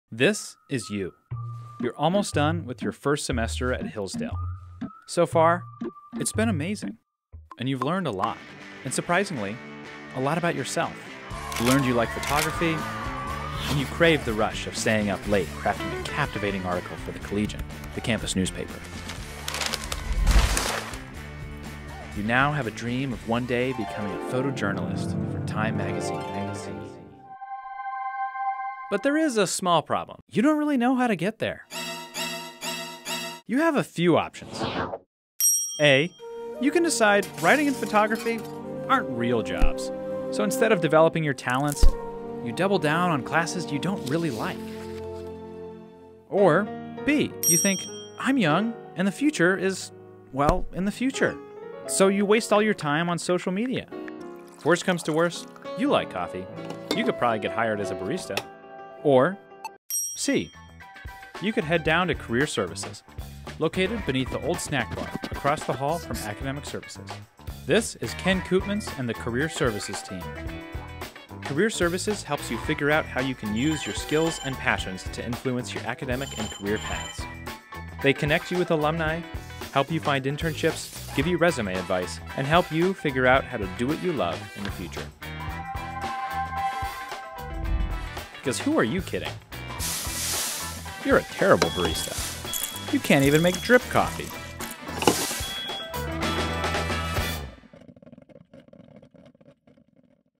Career Services | Conquering Freshman Year
This is Career Services.

Located in the lower level of the Knorr Student Center, Career Services is your best resource for job and internship search, as well as professional development.